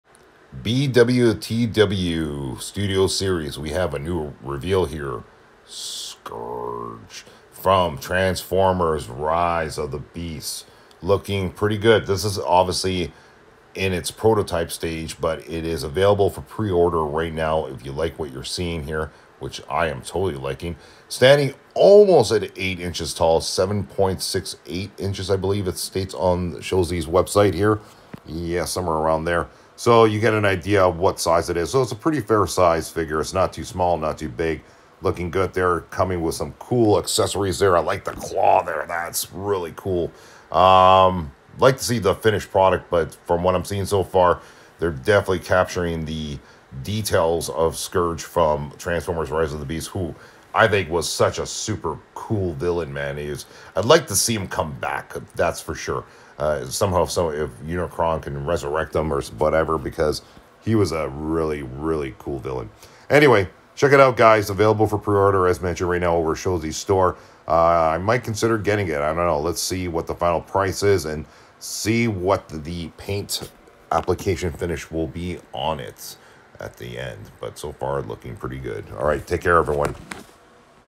New transformers rise of the beasts scourge action figure by BW TW-1031 Studio Series revealed
Available for preorder here